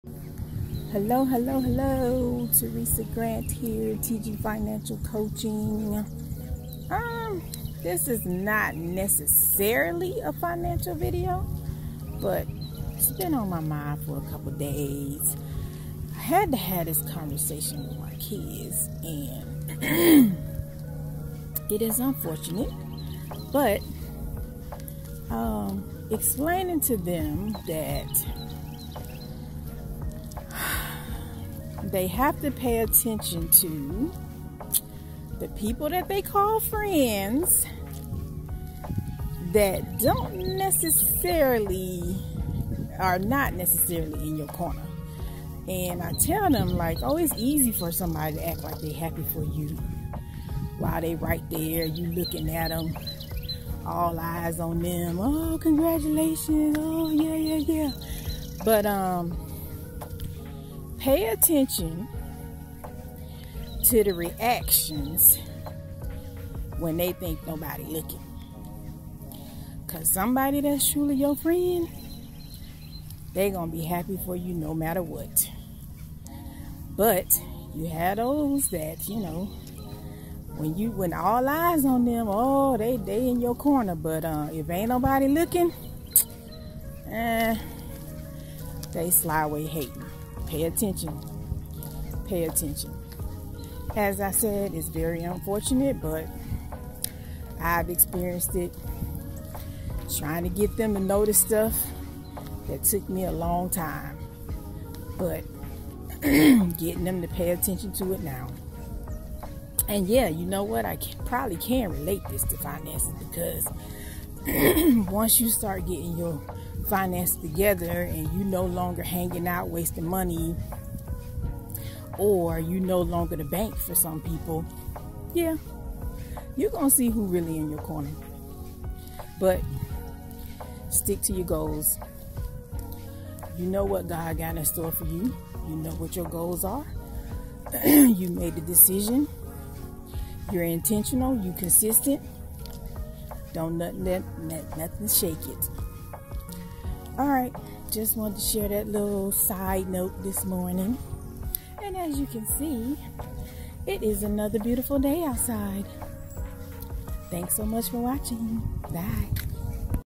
It's Very Unfortunate, But It Happens! Don't Let Them Shake You! Stick To Your Goals!
You Are Celebrating! Something Great Just Happened For You! But Your FRIEND. Pay Attention to the reaction, facial expressions, words of those friends when they think no one is looking or you are not around!

Unfortunate. Unbelievable...But True!